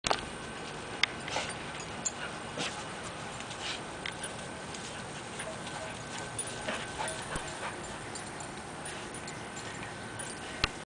The Dogs Play With Eachother
My dogs play witheachother in the front yard